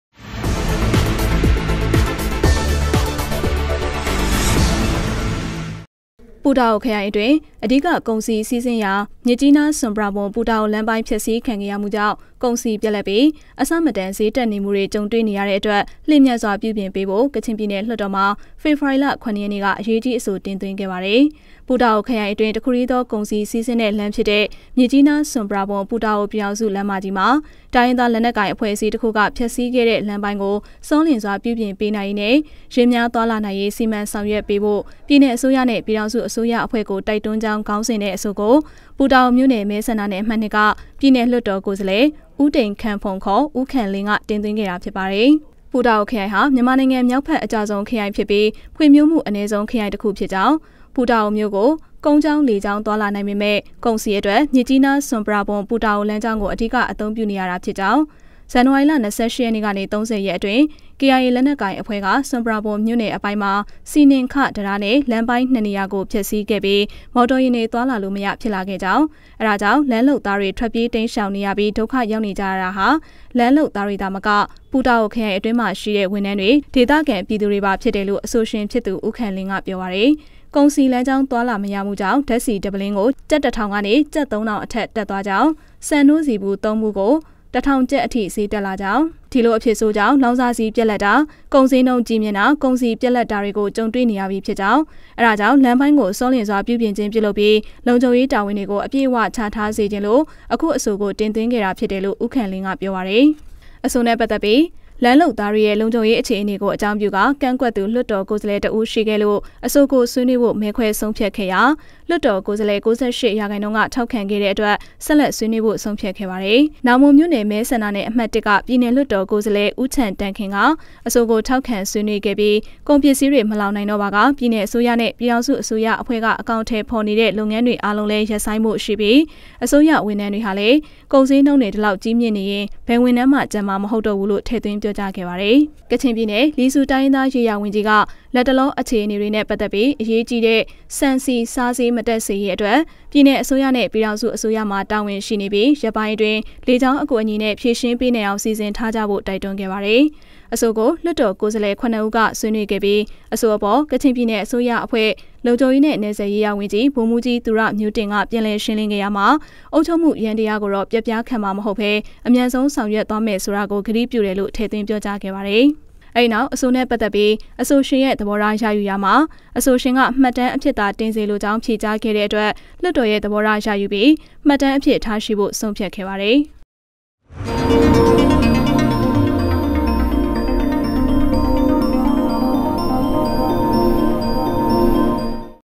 ျမစ္ၾကီးနား-ဆြမ္ပရာဘြမ္-ပူတာအိုလမ္းပိုင္း ဖ်က္ဆီးခံခဲ့ရမႈေၾကာင့္ ကုန္စည္ျပတ္လပ္ျခင္းမ်ား ၾကံဳေတြ႕ေနရ
ျမစ္ၾကီးနား-ဆြမ္ပရာဘြမ္-ပူတာအိုလမ္းပိုင္း ဖ်က္ဆီးခံခဲ့ရမႈေၾကာင့္ ကုန္စည္ျပတ္လပ္ျခင္းမ်ား ၾကံဳေတြ႕ေနရသည့္အတြက္ လ်င္ျမန္စြာျပဳျပင္ေပးရန္ ျပည္နယ္လႊတ္ေတာ္တြင္ အေရးၾကီးအဆိုတင္
ပူတာအိုခရိုင္အတြင္း အဓိကကုန္စည္စီးဆင္းရာ ျမစ္ၾကီးနား-ဆြမ္ပရာဘြမ္-ပူတာအို လမ္းပိုင္းဖ်က္ဆီးခံခဲ့ရမႈေၾကာင့္ ကုန္စည္ျပတ္လပ္ၿပီး အဆမတန္ေစ်းတက္ေနမႈေတြ ၾကံဳေနရတာေၾကာင့္ လ်င္ျမန္စြာျပဳျပင္ေပးဖို႔ ကခ်င္ျပည္နယ္ လႊတ္ေတာ္မွာ ေဖေဖာ္၀ါရီ ၇ ရက္ေန႔က အေရးၾကီးအဆိုတင္သြင္းခဲ့ပါတယ္။